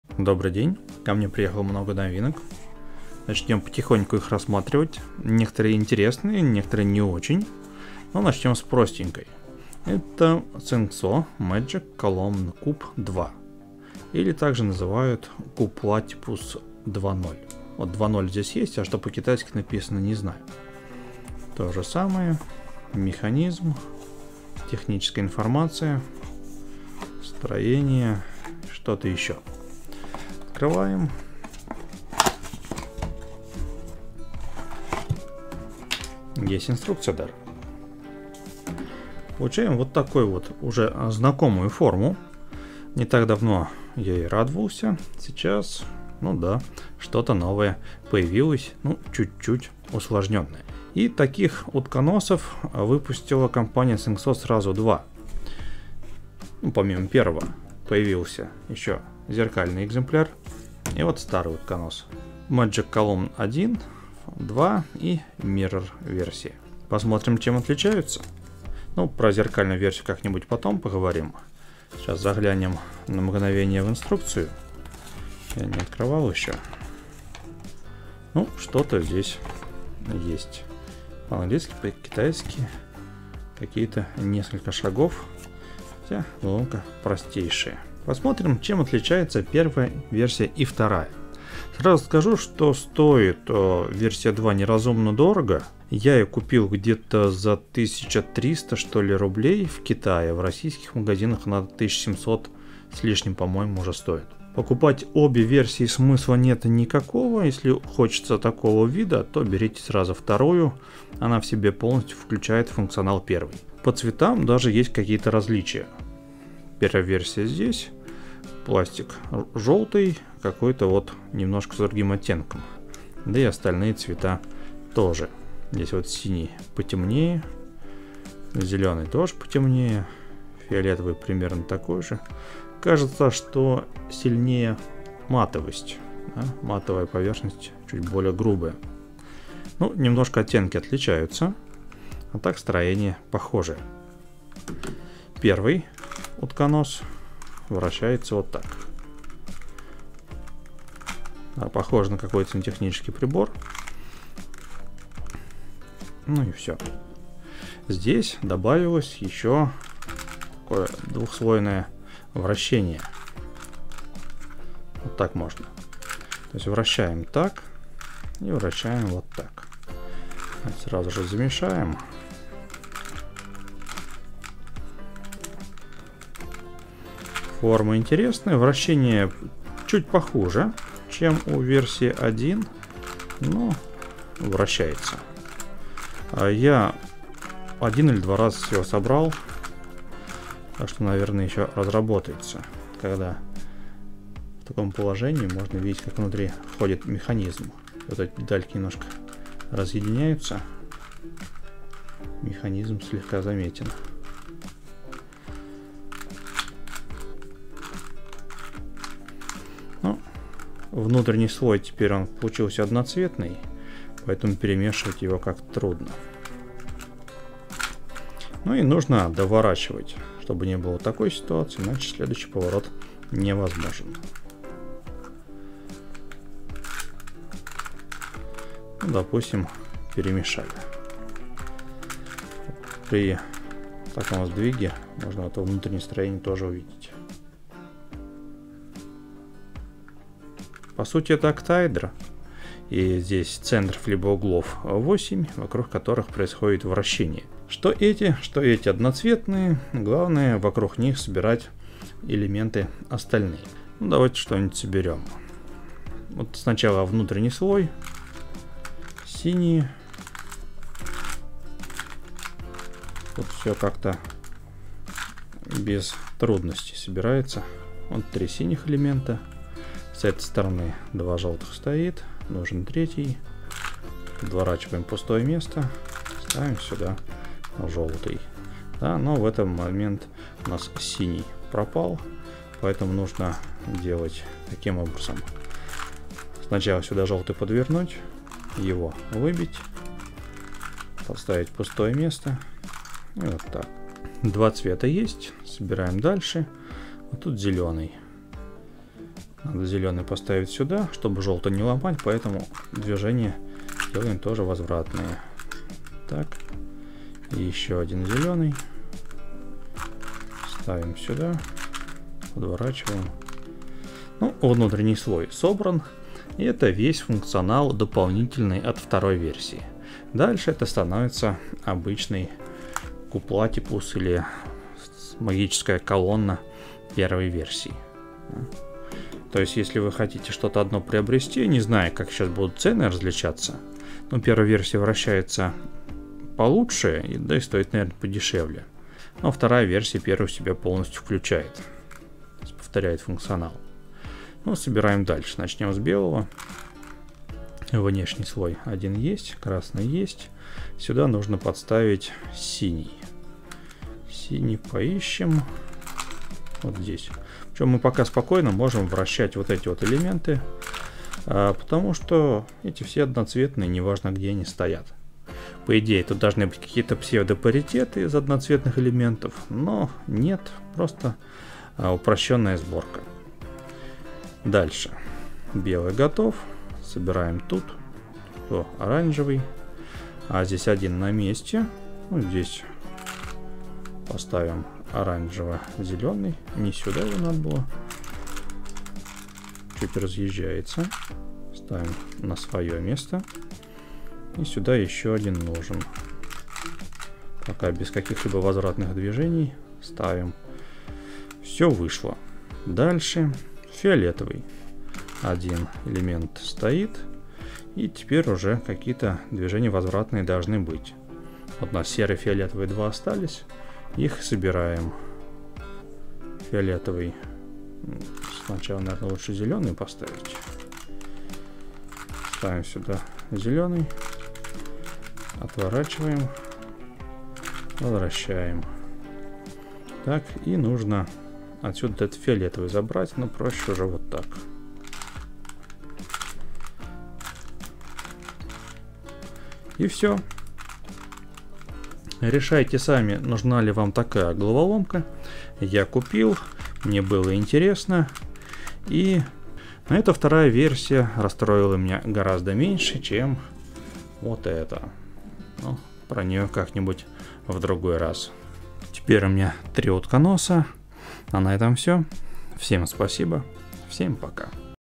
Unboxing №405 Sengso Magic Column 2 | Странный октаэдр 2 | Q-Platypus 2.0 | Утконос 2
Вторая версия магнитной нестандартной головоломки от Сенгсо. Сложность слегка увеличена.